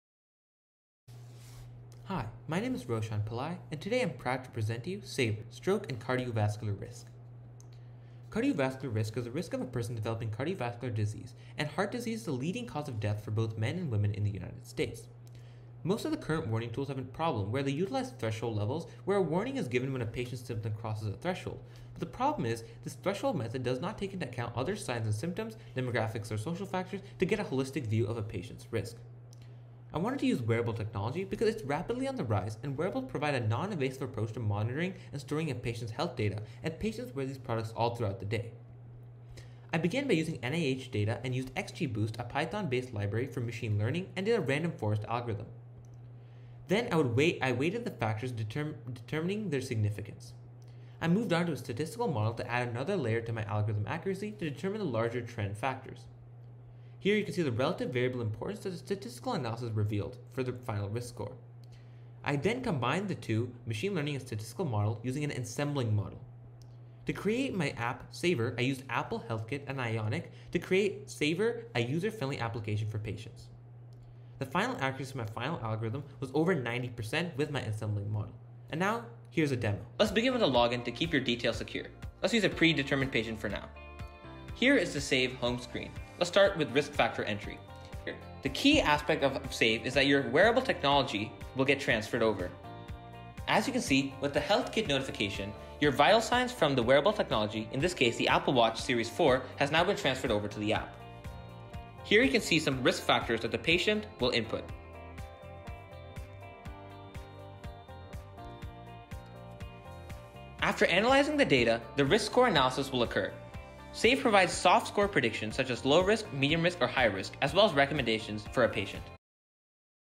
SAVR- Stroke and Cardiovascular Risk Assessment using Wearable Technology
Presented By RoshanPillai, BASIS Scottsdale

Cardiovascular risk is the risk of a person developing cardiovascular diseases, often sudden. Heart disease is the leading cause of death for both men and women. Most of the current warning tools utilize the threshold level wherein, a warning is given when a patient’s symptom crosses a threshold. Unfortunately, this threshold method does not take into account other signs and symptoms demographic, or social factors, nor does it attain a 360-degree view of a patient’s
risk. Cardiovascular issues can appear rapidly and without warning. Heart attacks (Myocardial Infarction) and Strokes (Cerebrovascular Accidents) can cause irreversible damage and focus on prevention or early diagnosis is still needed.

Also, the current risk calculators and available tools do not provide results based on input from wearables/ live data and also do not incorporate the latest technology that’s available to the users to include information from their medical records. I created an app that utilizes an interoperable based approach where a patient/user can take their everyday data from a wearable, integrate the information available on their health app/health records and with user input, including their reported medical symptoms to screen and find out their relative risk for stroke/cardiovascular disease.

My design incorporated integration with Wearable Technology, the ability to integrate live data, and allow user input, as well as the ability to access health records (via PHI: Protected Health Information) in a HIPAA compliant manner to protect patient privacy. I formulated a hierarchical project architecture to create this application. My first layer was the data collection layer. Once I had obtained the data, I converted the data to .csv files for easier analysis. The next layer was the analytical model layer where I utilized machine learning, created a statistical model, and then combined the two models to create my algorithm. Once my algorithm generated both a viable and accurate structure for prediction, the next step was implementation. Lastly, I constructed an Intelligence Layer to access Wearable Technology as well as create SAVR (Stroke and Vascular Risk), my mobile application. For my machine learning algorithm, my unique approach was to mesh both statistical analyses through the use of Ensembling Models. The machine learning method used was XGBoost, an open-source, R and Python-based software library which is a versatile tool made to work through most regression, classification, and ranking problems as well as user-built objective functions. I chose XGBoost because it’s a library designed and optimized for boosted tree algorithms which are specifically tuned towards training an algorithm for software functionality. Also, XGBoost is known for its model performance/ accuracy and execution speed; both critical for this project. I then did the statistical model to add another layer to my algorithm accuracy. I did a linear regression and created a statistical model algorithm using Python. The algorithm plots all of its analyzed data then finds the most accurate linear line to its predictions. Then, I performed a linear regression to find out how my application should weigh different factors. After I generated both a machine learning model and a statistical model, I used Ensembling Models to combine the two. The combined algorithm revealed how different risk factors must be designed to create an accurate prediction for a patient.

Lastly, to build the SAVR Application, I used Ionic, a hybrid app development platform. Ionic is an open-source Java-Script-based Language. With Ionic, I combined the Analytical Model Layer and the Intelligence Layer to create the Presentation Layer, where the user could input their data and get a predicted Risk Score. This was SAVR, the wearable app that I hope that one day will be a tool that helps save lives.